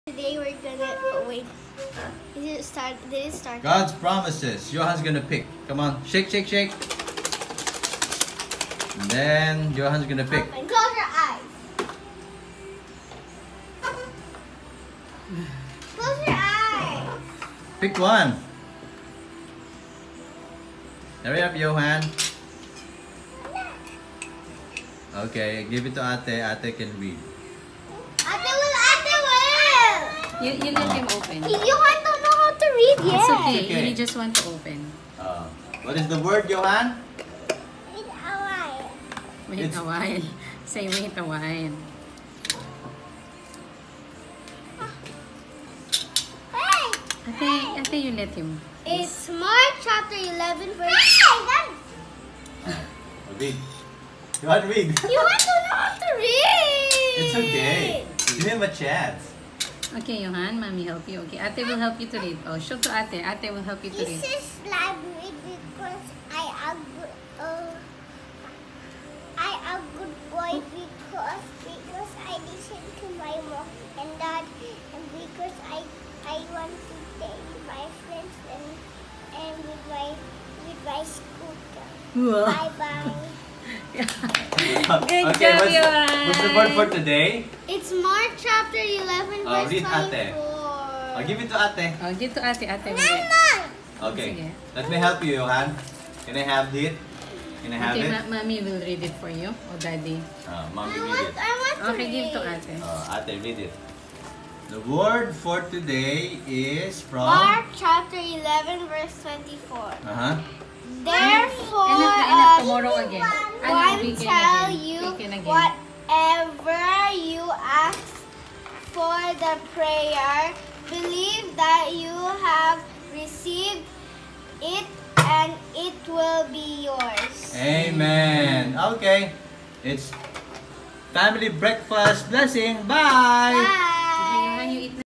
Family Breakfast Blessings. Word for today: Mark 11:24
“Therefore I tell you, whatever you ask for in prayer, believe that you have received it, and it will be yours.”
‭‭Mark‬ ‭11:24‬ ‭NIV‬‬